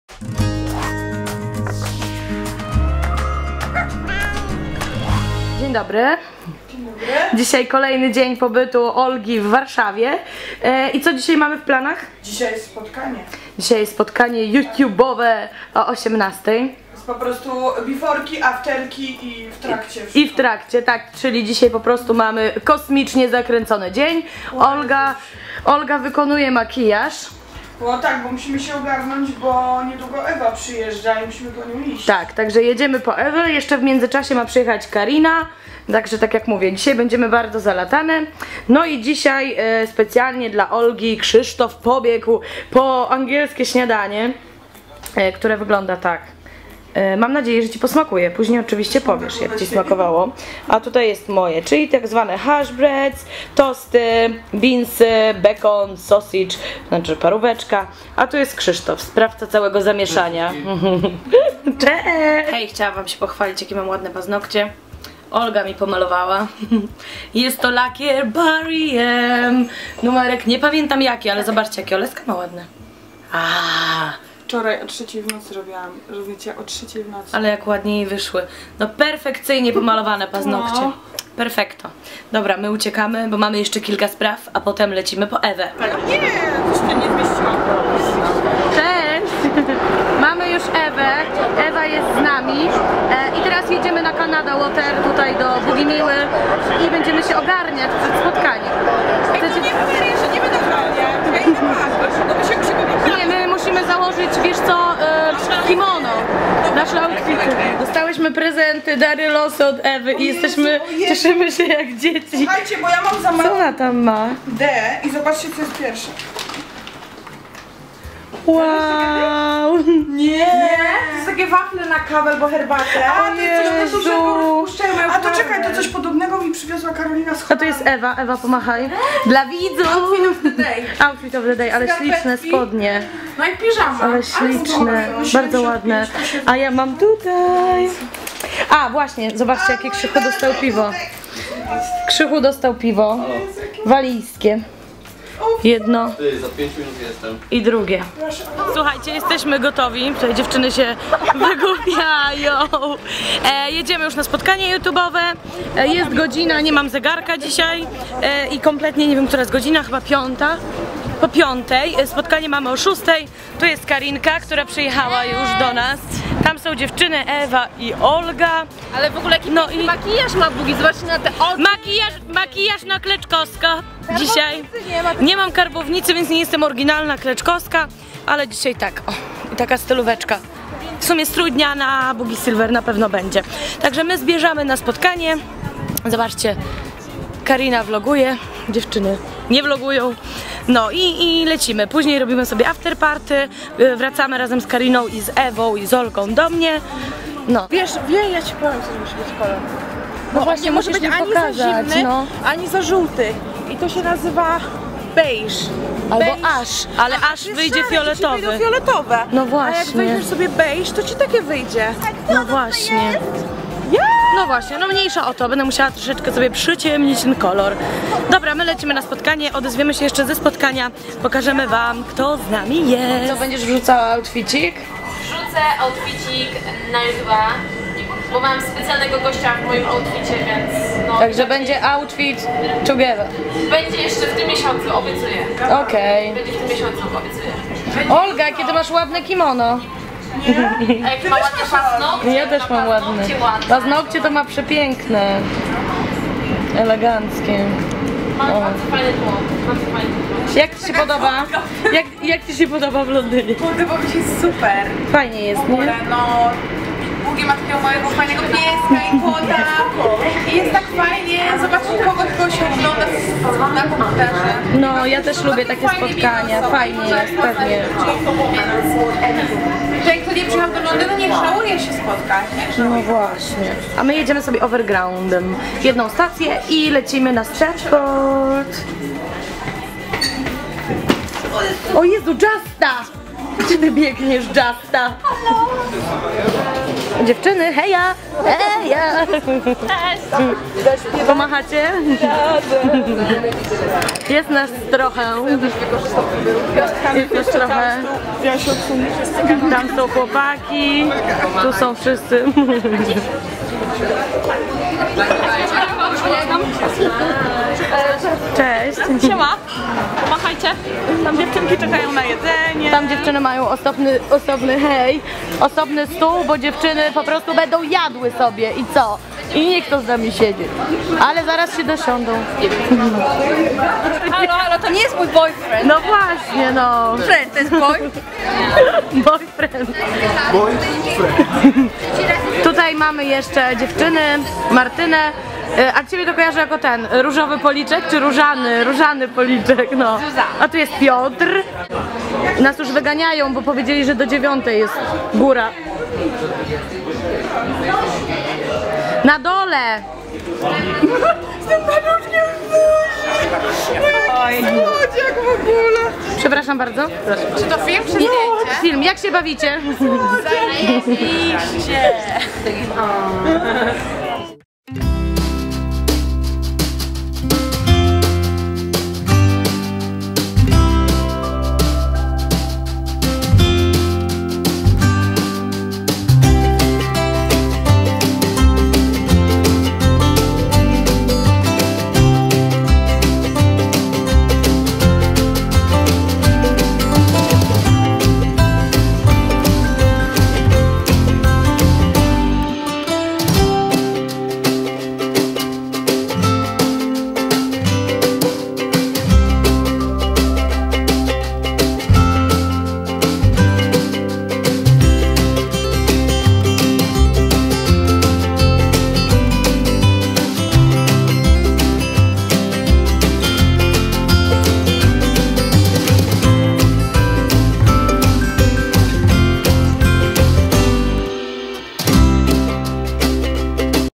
THEOLESKAAA W LONDYNIE 2 - DZIEN Z DZIEWCZYNAMI - SPOTKANIE YOUTUBOWE !
Dni z naszego codziennego zycia !

*Bloopers
*Extras

♡ T W I T T E R @boogiesilver
♡ I N S T A G R A M @boogiesilver

♡ Napisz do mnie/ Send me mail:
PAULA RUDEK
SKR.17 UL. BACIECZKI 212
15-695 BIALYSTOK 35